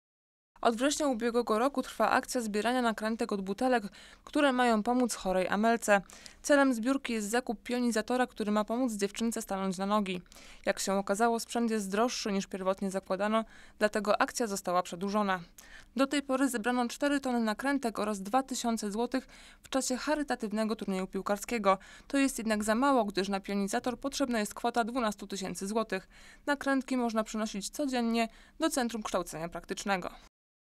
TKB - Nakrętki dla Amelki - 26.05.2017
Od września ubiegłego roku trwa akcja zbierania nakrętek od butelek, które mają pomóc chorej Amelce. Celem zbiórki jest zakup pionizatora, który ma pomóc dziewczynce stanąć na nogi.

Jak się okazało, sprzęt jest droższy niż pierwotnie zakładano, dlatego akcja została przedłużona. Do tej pory zebrano 4 tony nakrętek oraz 2000 zł w czasie charytatywnego turnieju piłkarskiego. To jest jednak za mało, gdyż na pionizator potrzebna jest kwota 12 tys. zł.

Nakrętki można przynosić codziennie do Centrum Kształcenia Praktycznego.